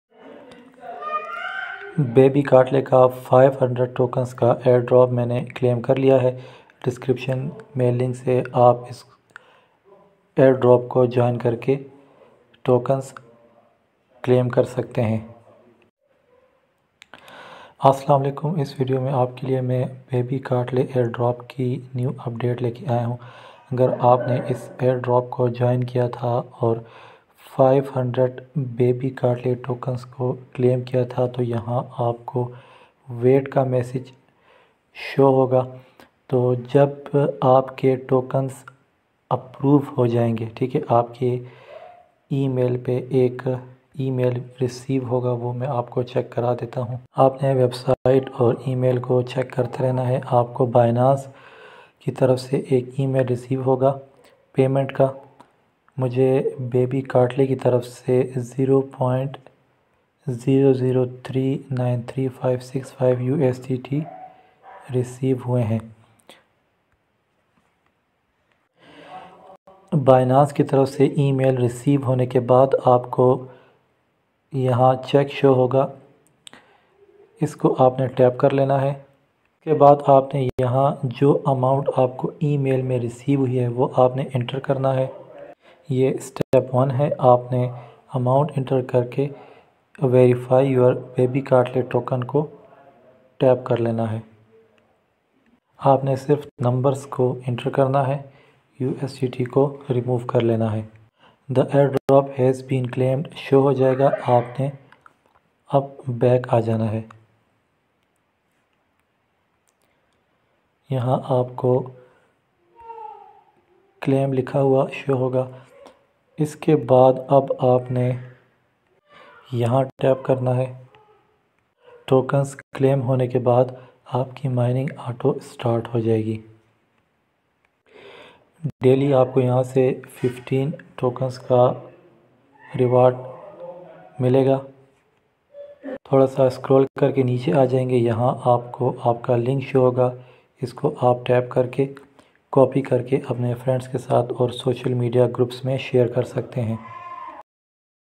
Binance Email Received|500 $BCatly Tokens Claimed|BabyCatly Airdrop New Update|Technical Shahnavi
BabyCatly Airdrop New Update

Binance Email Received

500 $BCatly tokens have been claimed

I'm on Instagram as @ubaidullahshahnavee. Install the app to follow my photos and videos.